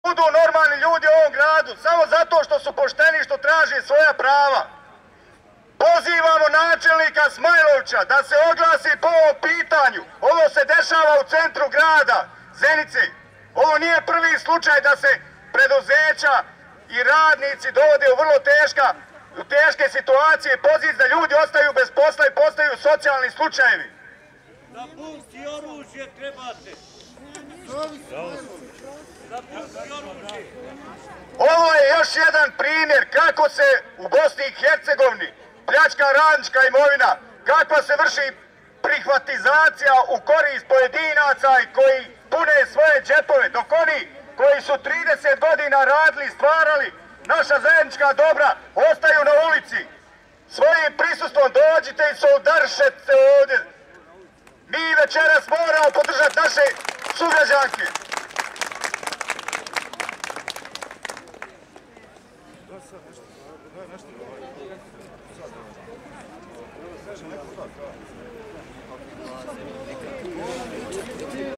snimak okupljanja rodbine radnika RK Bosna Zenica
U večernjim satima ispred RK Bosna Zenica desilo se spontano okupljanje rodbine, prijatelja i građana s namjerom da daju podršku radnicima u njihovim zahtjevima.
Okupljenim građanima obratio se Hasan Kreho aktivista udruženja "Akcija Građana" pozivajući i ostale građane da podrže svoje komšije.
Nešto iza 21,00 dogovoreno je da se okupljeni vrate svojim kućama zbog hladnoće a da  sutra u 12'00 ponovo nastave sa podrškom.

Dogovoreni su i zahtjevi građana i to:

1. Pod HITNO da se dostavi hrana, struja, voda i lijekovi radnicma

2. da se omoguće pregovori sa "vlasnikom" Jasminom Selmanagićem ili predstavnikom preduzeća "BEOKOM"